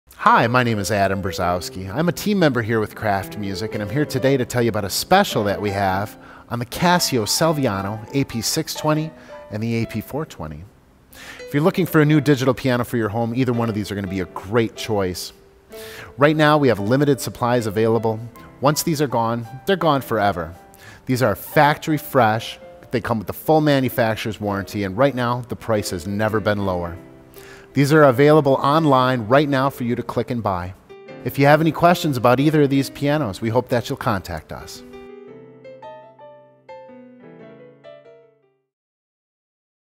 Kraft Music - SPECIAL OFFER on Casio Celviano AP-620 & AP-420 Digital Pianos
See Kraft Music's SPECIAL OFFERS on Casio Celviano AP-620 & AP-420 console digital pianos:

Blow-Out Sale! Get a great deal on the Casio AP-620 and Casio AP-420 digital pianos from Kraft Music. We have a special sale on these new pianos at prices never this low. Get even more value by purchasing one of our exclusive BUNDLES that include great accessories to go with your new Celviano new home piano, such as a brass piano lamp and a pair of quality stereo headphones for private practice. Be sure to check out our "PLUS" BUNDLE, that is similar to our COMPLETE HOME BUNDLE with the addition of a subwoofer speaker. This subwoofer enhances the Celviano's on-board speaker system and adds power and fullness to the sound of the instrument. You'll be amazed by how good your Casio home piano sounds when connected to the additional sub!